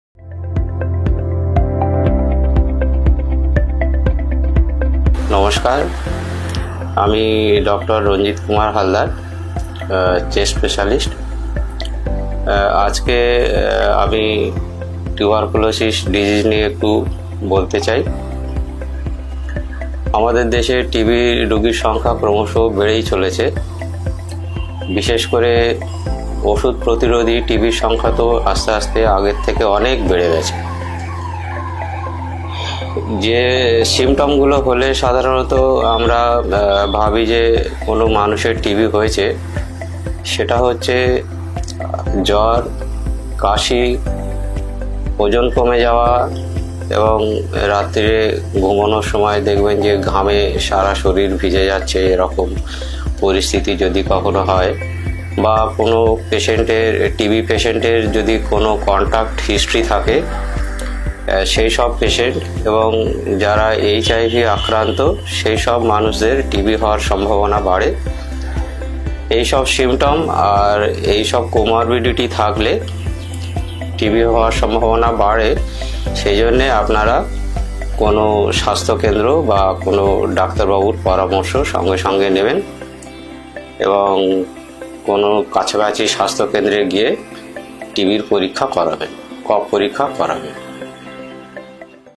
Highlighting *Tuberculosis* symptoms at Samaritan, Dr. Ranjit Kumar Halder
**Dr. Ranjit Kumar Halder**

Highlighting *Tuberculosis* symptoms at Samaritan, Dr. Ranjit Kumar Halder addresses the nationwide concern. With fever, recurring cough, weight loss, and drug-resistant strains prevalent, he advises prompt consultation with the nearest doctor and hospital for anyone experiencing these symptoms. 🩺🔍